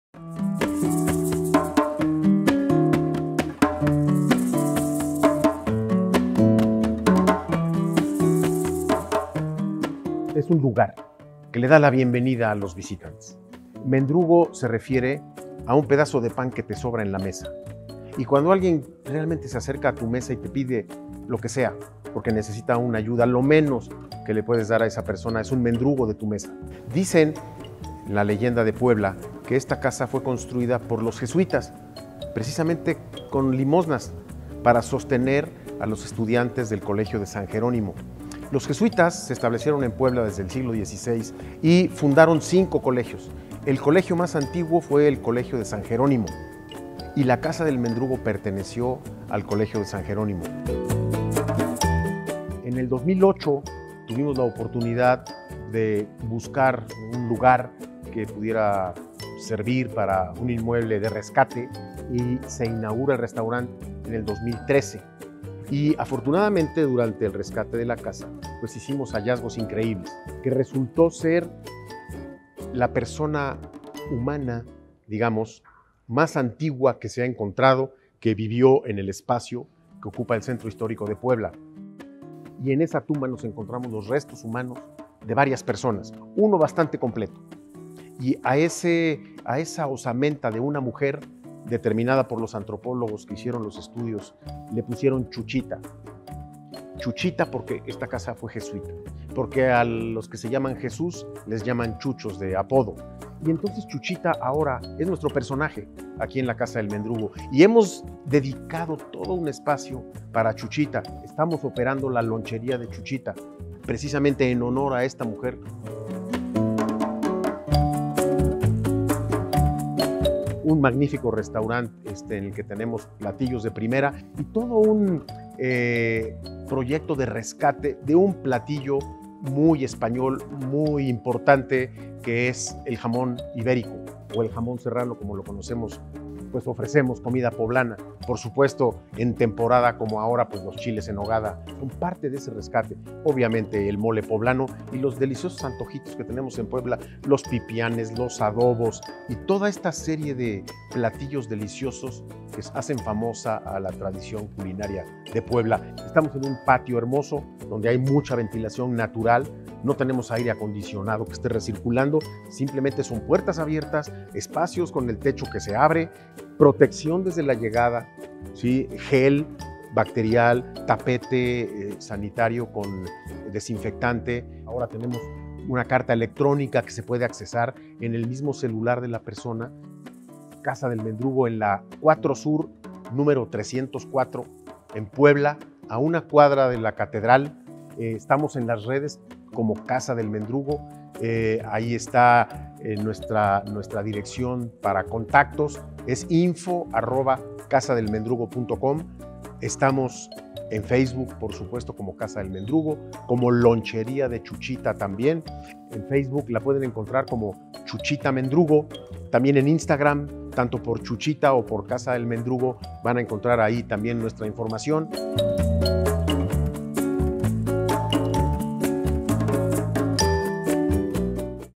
La Casa del Mendrugo - Un espacio con una de las historias más divinas de Puebla
La historia de La Casa del Mendrugo es impresionante para aquellos que la escuchan.
Una gran casona antigua que fue restaurada, y en medio de dicha restauración fue hallada "Chuchita" la habitante más antigua de Puebla.

Visítanos y conoce los maravillosos espacios que tiene La Casa del Mendrugo para ti.